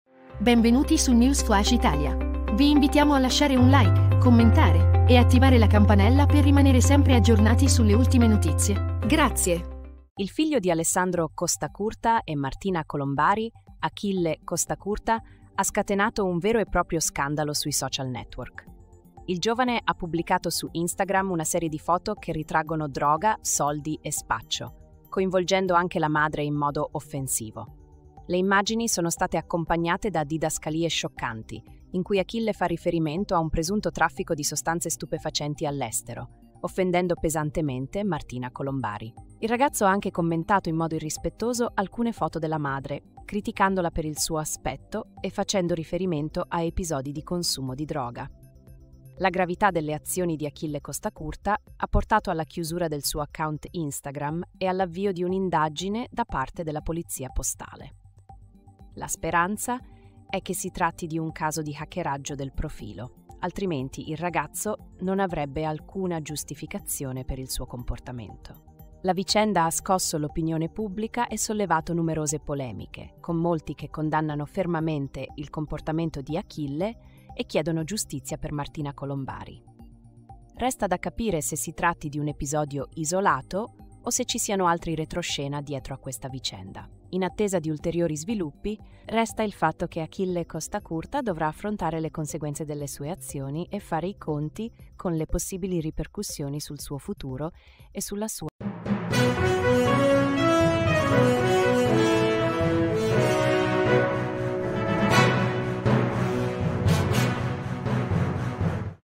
SCANDALO SOCIAL: ACHILLE COSTACURTA CONDIVIDE FOTO DI DROGA E SPACCIO
Il figlio di Alessandro Costacurta e Martina Colombari, Achille Costacurta, ha scatenato un vero e proprio scandalo sui social network. Il giovane ha pubblicato su Instagram una serie di foto che ritraggono droga, soldi e spaccio, coinvolgendo anche la madre in modo offensivo.

Le immagini sono state accompagnate da didascalie scioccanti, in cui Achille fa riferimento a un presunto traffico di sostanze stupefacenti all'estero, offendendo pesantemente Martina Colombari. Il ragazzo ha anche commentato in modo irrispettoso alcune foto della madre, criticandola per il suo aspetto e facendo riferimento a episodi di consumo di droga.

La gravità delle azioni di Achille Costacurta ha portato alla chiusura del suo account Instagram e all'avvio di un'indagine da parte della Polizia Postale. La speranza è che si tratti di un caso di hackeraggio del profilo, altrimenti il ragazzo non avrebbe alcuna giustificazione per il suo comportamento.

La vicenda ha scosso l'opinione pubblica e sollevato numerose polemiche, con molti che condannano fermamente il comportamento di Achille e chiedono giustizia per Martina Colombari. Resta da capire se si tratti di un episodio isolato o se ci siano altri retroscena dietro a questa vicenda.

In attesa di ulteriori sviluppi, resta il fatto che Achille Costacurta dovrà affrontare le conseguenze delle sue azioni e fare i conti con le possibili ripercussioni sul suo futuro e sulla sua immagine pubblica.